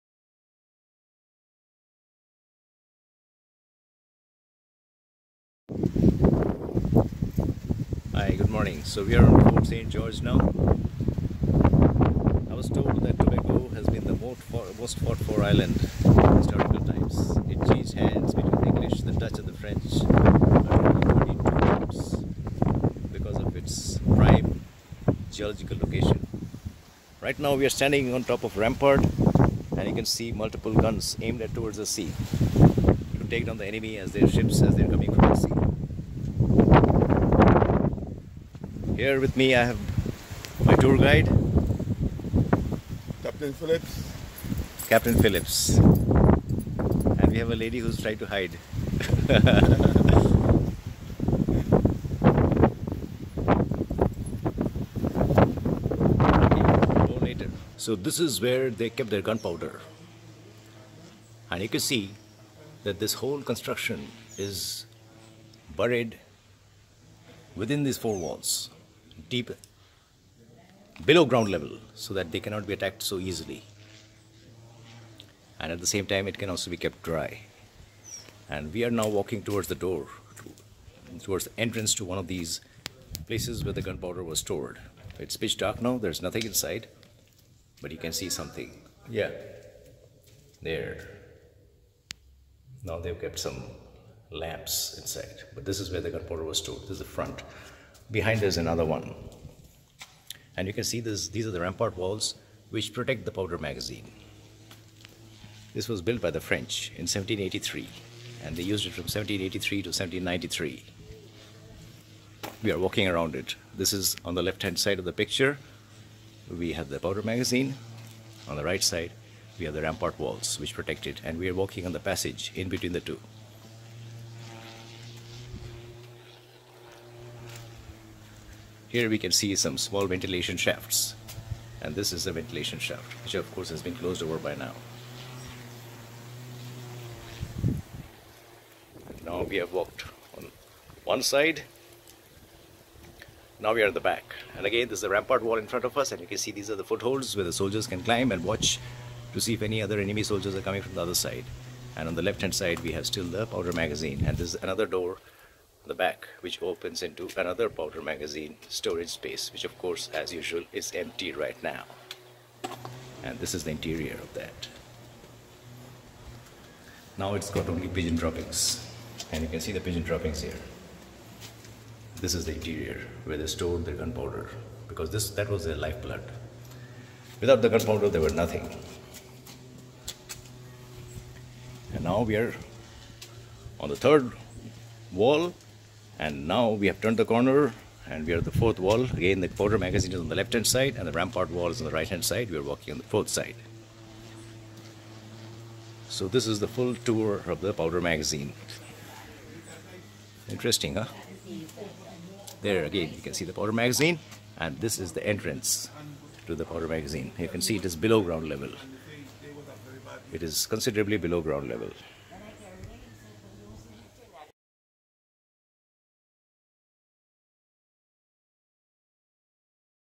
FORT KING GEORGE SCARBOROUGH TOBAGO - SANJOY SANYAL
Tour of Fort King George in Scarborough, Tobago, Trinidad and Tobago.
Did you know this Fort changed hands between the British, French and Dutch 39 times?
Guided by Capt Phillips.
Video captured with my Samsung 8 / 12 Megapixel camera, and edited with Windows Movie Maker.